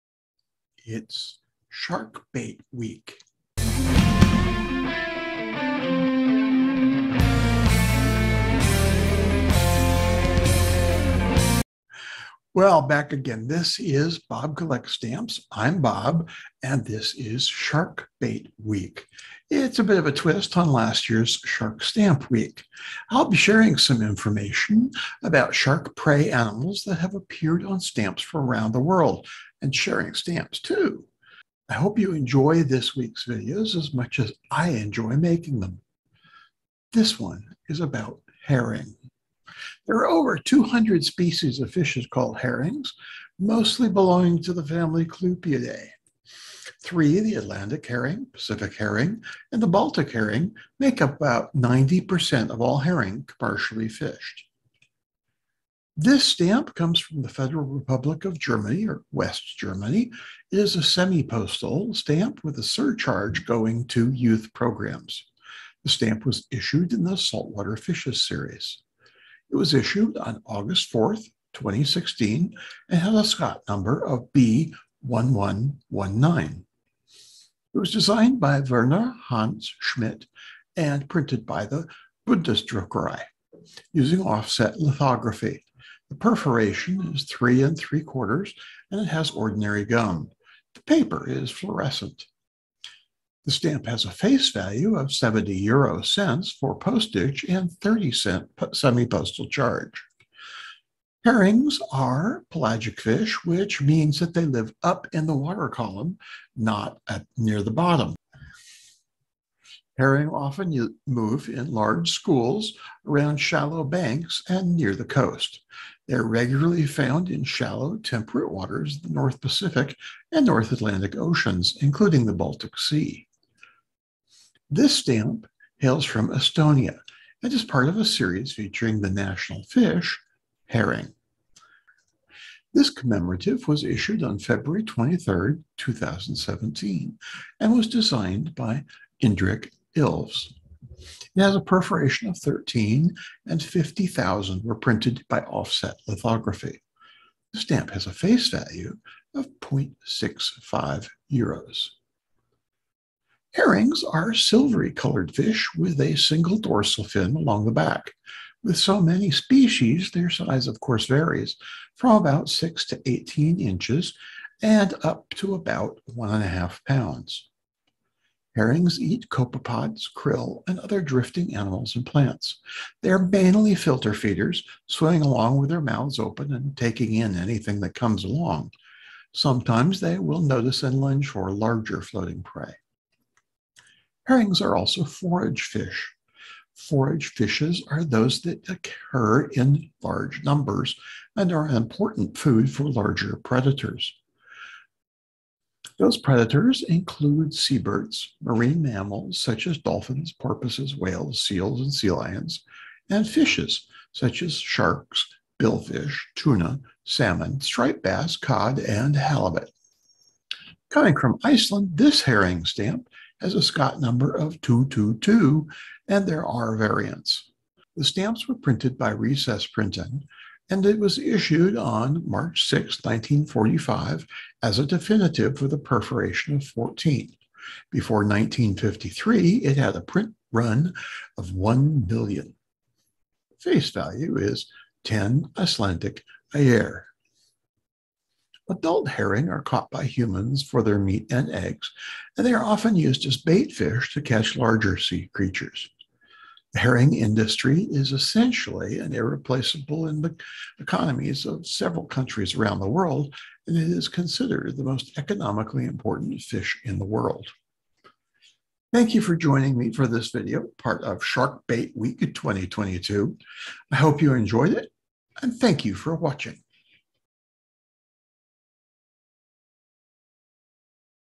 Shark *Bait* Week: Herring and stamps
It's Shark *Bait* Week! All about the animals that sharks prey upon, and the stamp they appear on. This time, it's about Herring.